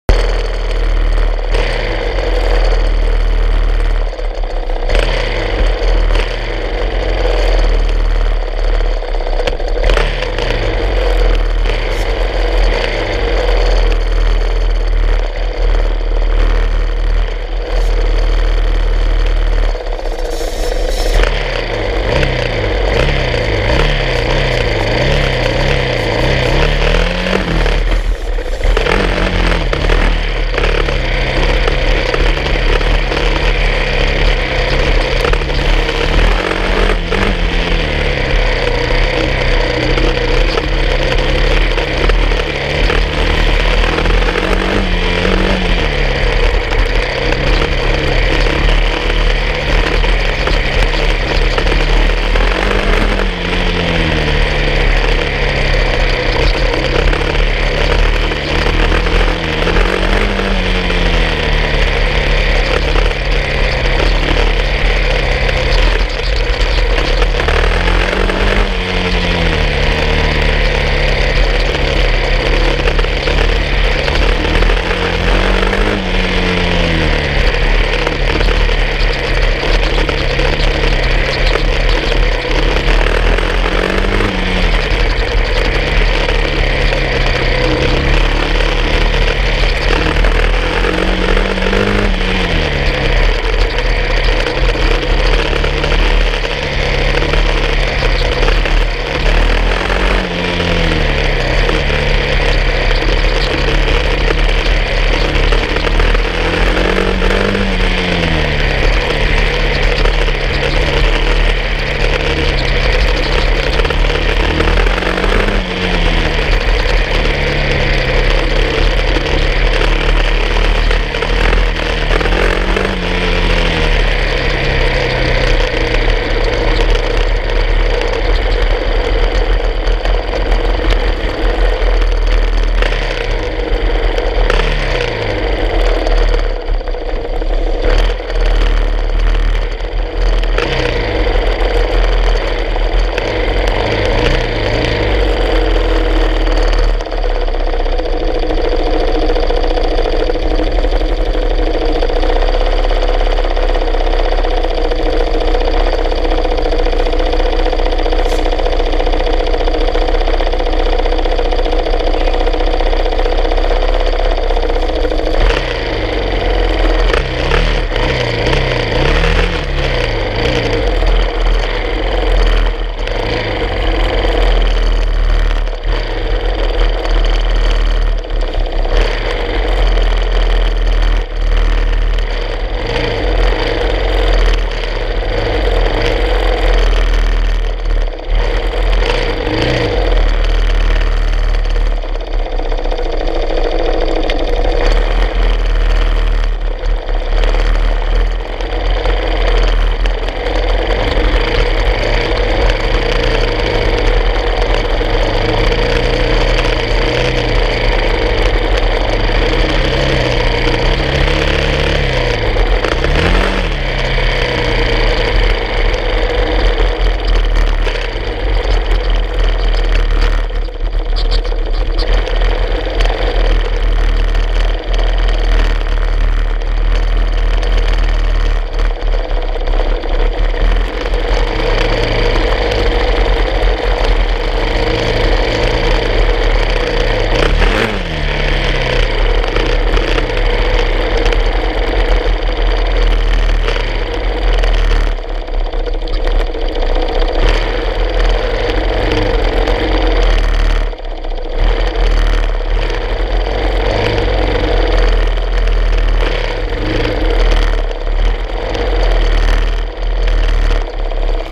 Open B heat cedar lake spwy
Flat track indoors with a sportster rubbing is racing even if it's with the  wall